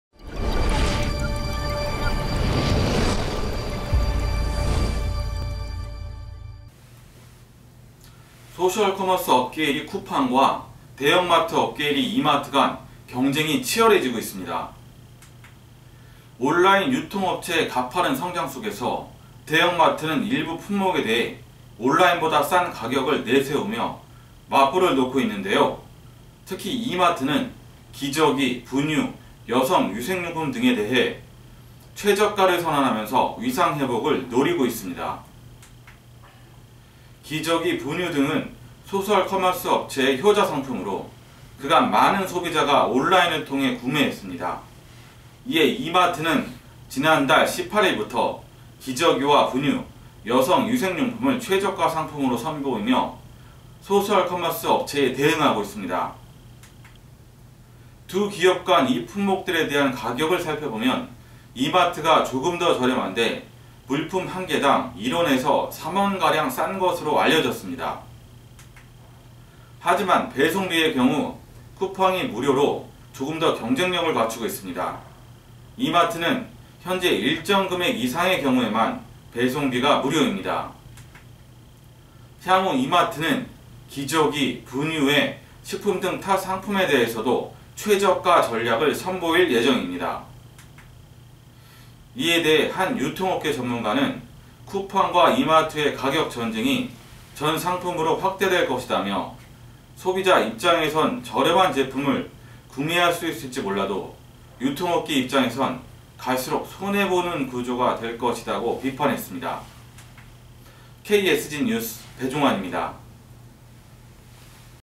이마트-쿠팡, 경쟁구도 격화
소셜커머스 업계 1위 쿠팡과 대형마트 업계 1위 이마트 간 경쟁이 치열해지고 있습니다.

온라인 유통업체의 가파른 성장속에서 대형마트는 일부 품목에 대해 '온라인보다 싼 가격'을 내세우며 맞불을 놓고 있는데요, 특히 이마트는 기저귀, 분유, 여성위생용품 등에 대해 최저가를 선언하면서 위상 회복을 노리고 있습니다.

기저귀, 분유 등은 소셜커머스 업체의 효자 상품으로 그 간 많은 소비자가 온라인을 통해 구매했습니다. 이에 이마트는 지난달 18일부터 기저귀와 분유, 여성위생용품을 최저가 상품으로 선보이며 소셜커머스 업체에 대응하고 있습니다.

두 기업 간 이 품목들에 대한 가격을 살펴보면 이마트가 조금 더 저렴한데, 물품 한 개당 1원에서 3원 가량 싼 것으로 알려졌습니다.

하지만 배송비의 경우 쿠팡이 무료로 조금 더 경쟁력을 갖추고 있습니다 이마트는 현재 일정금액 이상의 경우에만 배송비가 무료입니다.

향후 이마트는 기저귀, 분유 외 식품 등 타 상품에 대해서도 최저가 전략을 선보일 예정입니다.

이에 대해 한 유통업계 전문가는 "쿠팡과 이마트의 가격전쟁이 전 상품으로 확대될 것이다"며 "소비자 입장에선 저렴한 제품을 구매할 수 있을지 몰라도 유통업계 입장에선 갈수록 손해 보는 구조가 될 것이다"고 비판했습니다.